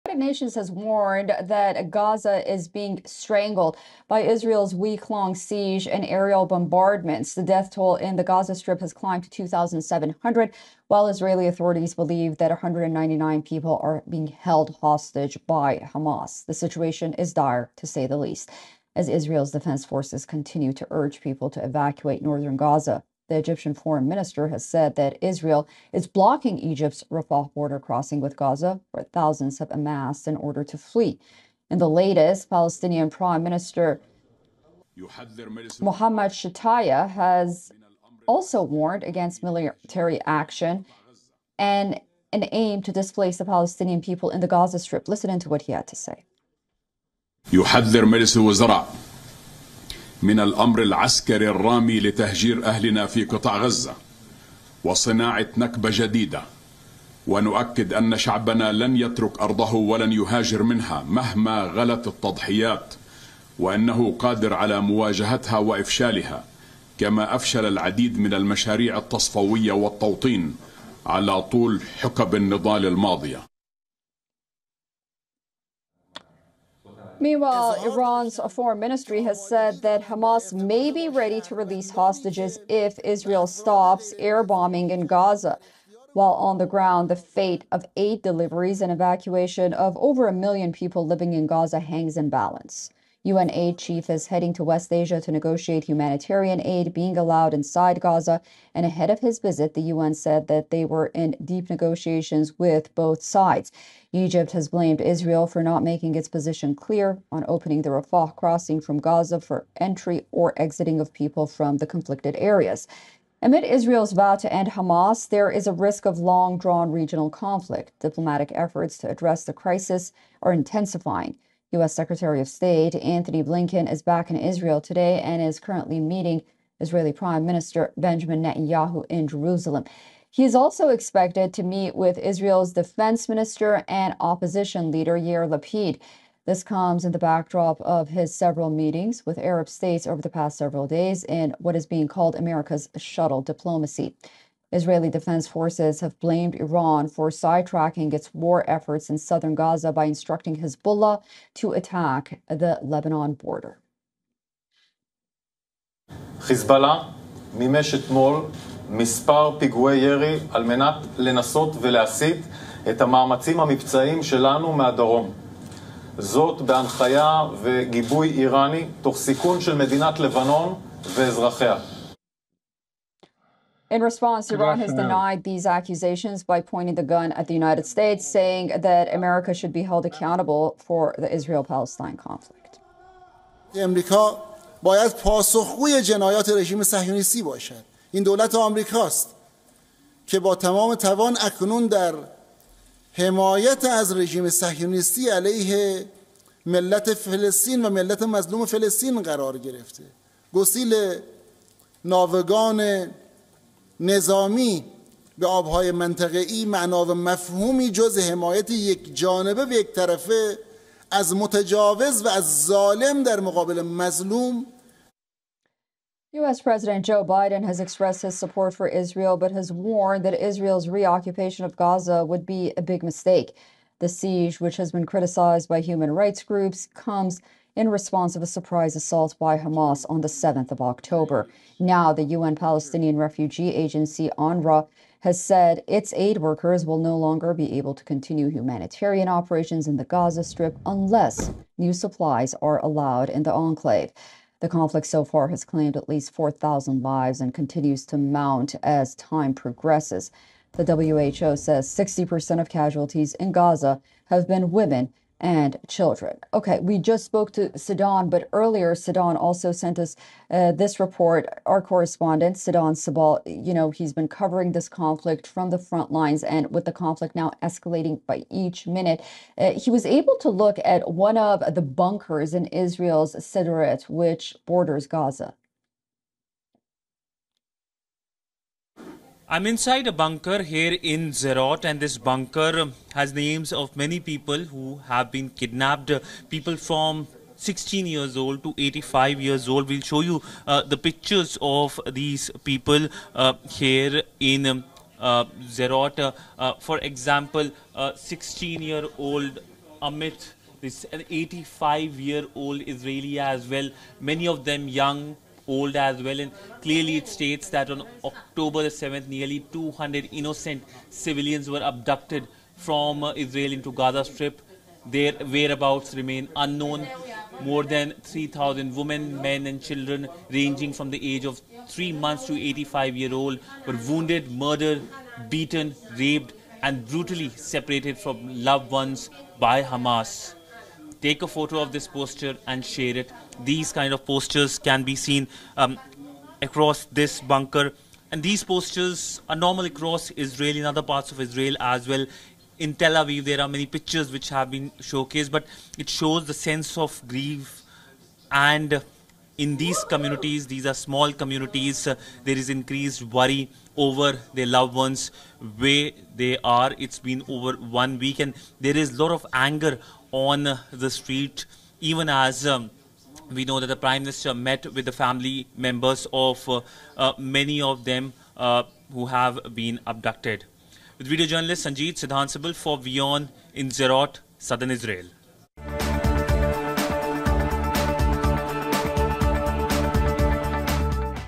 Israel-Palestine war: UN aid chief heads to Middle East to take forward Gaza aid talks | WION
Israel is now gearing up for a potential multi-front War days after a devastating onslaught from Gaza's Hamas regime. Over 1,200 Israelis have been killed in the Hamas attacks so far on the other hand 900 have been reported killed in Gaza which has come under intense bombardment from Israel's retaliatory strikes.